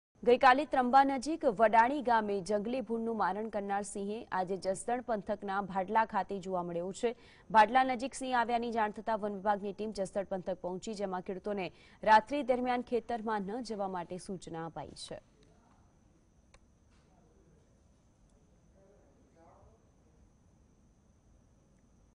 Rajkot : ત્રંબા નજીક વડાલી ગમે જંગલી ભૂંડનું મારણ કરનાર સિંહે દેખા દીધી
Gujarati news